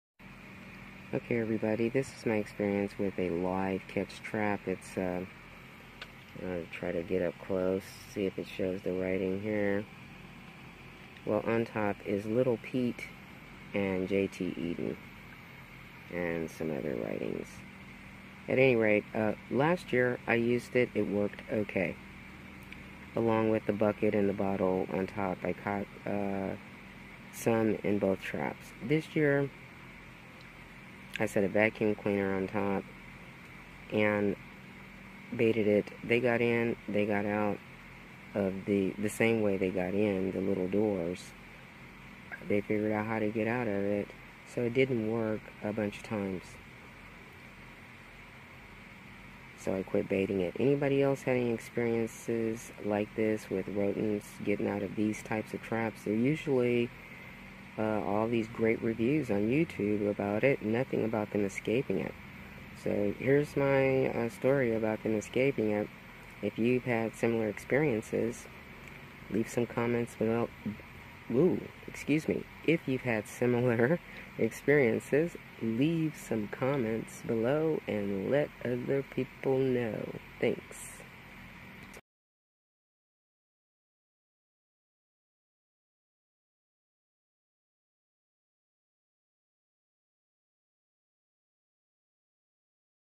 Mouse in the House it escaped live trap
Little Pete j t Eaton live catch mouse trap, tin cat, live trap, rat trap etc. Mouse escaped the trap.